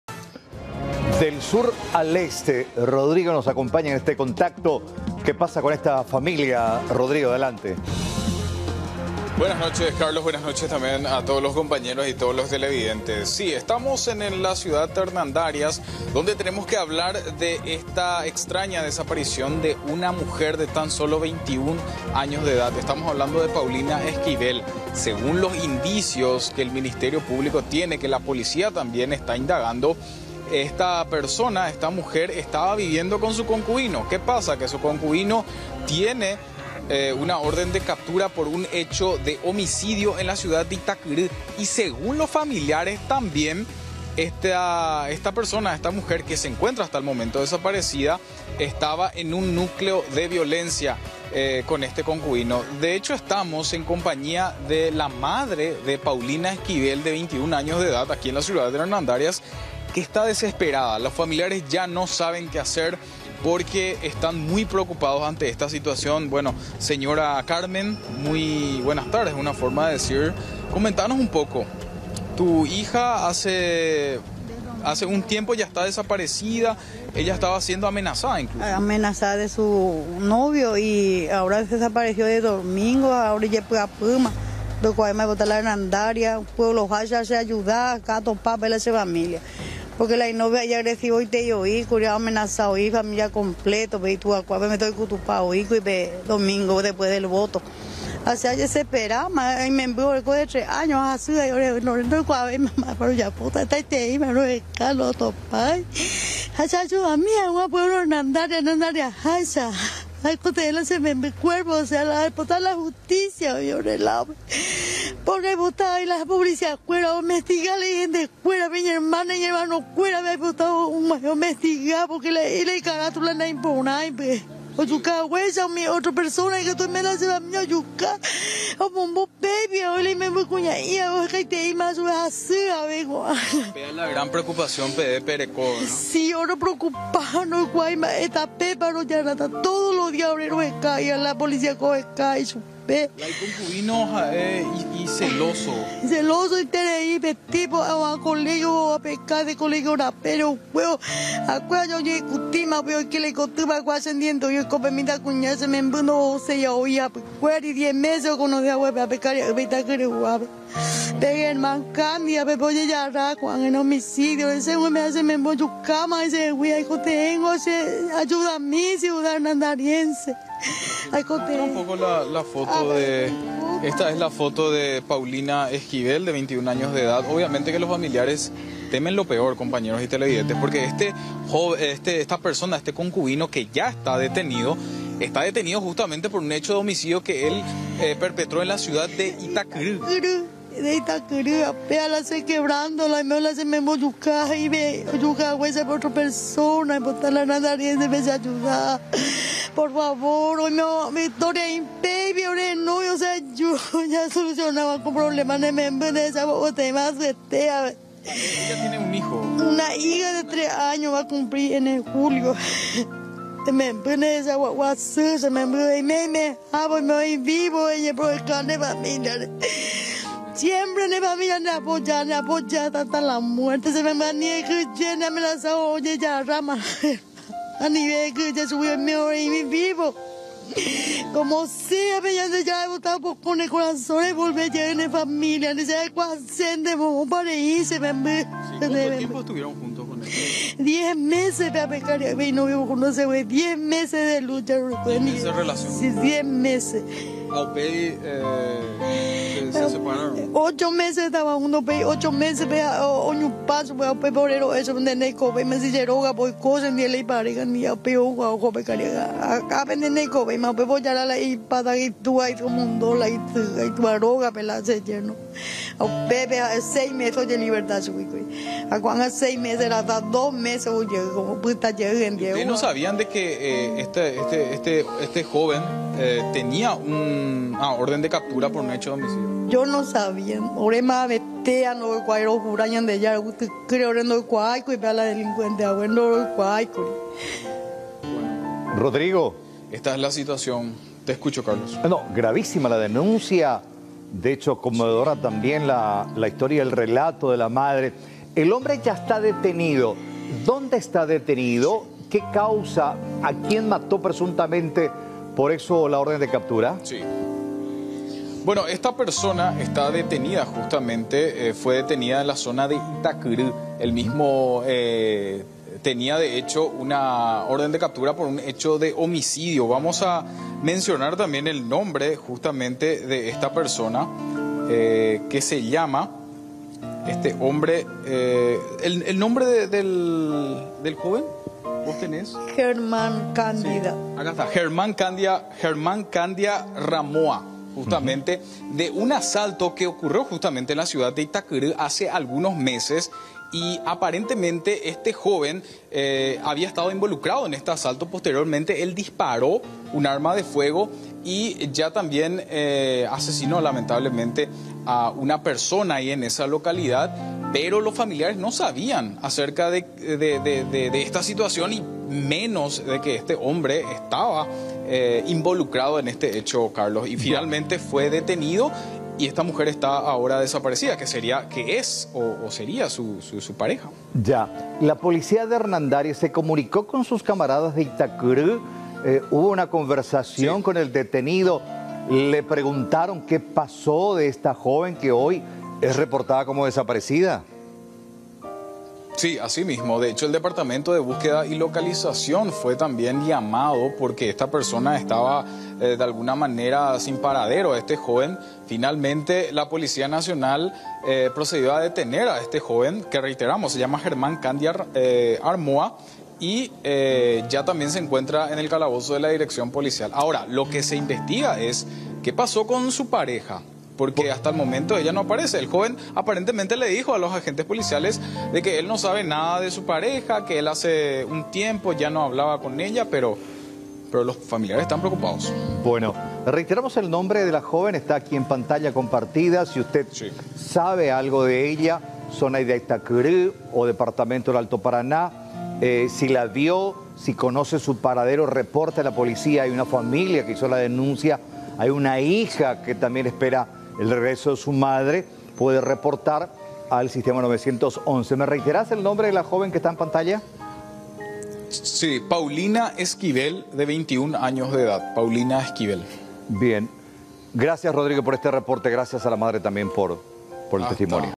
¿Dónde está Paulina?
En Hernandarias, Paulina Esquivel de 21 años se encuentra desaparecida.

La misma vivía con su concubino, quien tenía una orden de captura por homicidio. El mismo ya está detenido.

"Su novio es muy agresivo", comentó su madre.